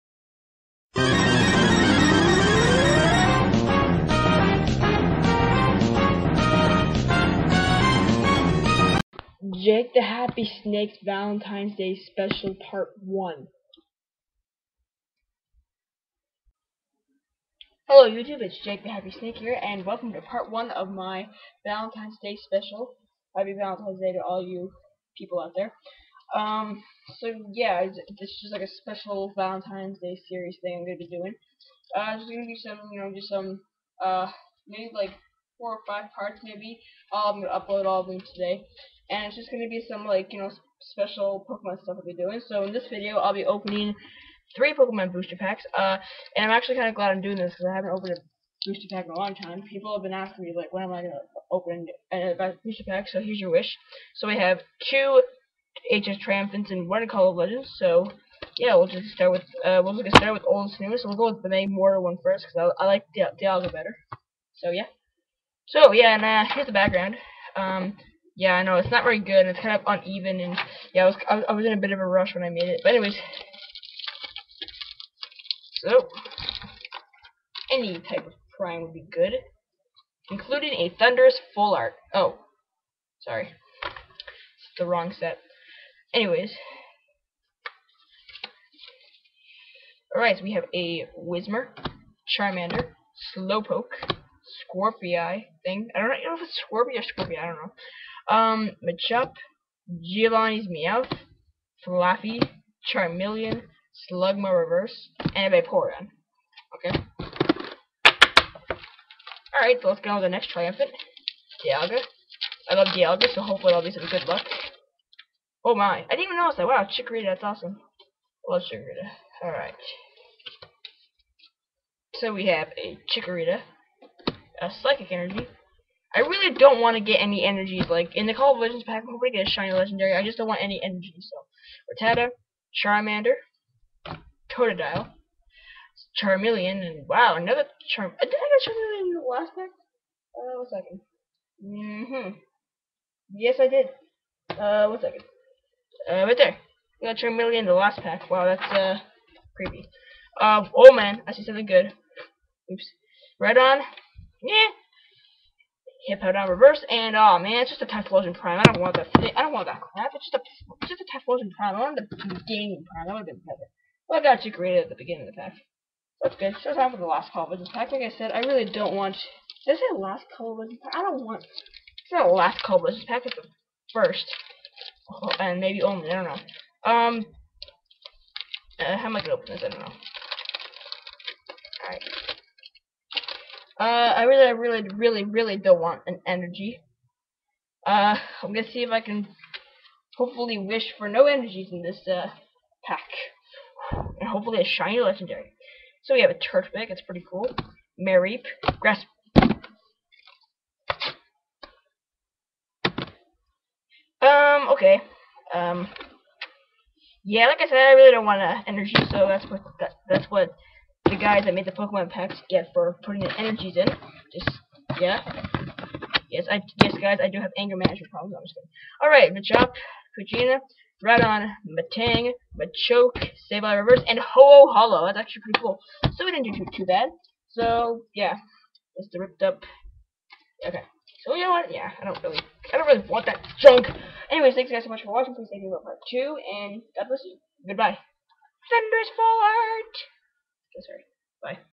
Jakethehappysnake's Valentines Day Special Part 1
Welcome To Part 1 Of My Valentines Day Special! In This Part, I Open 3 Pokemon Booster Packs!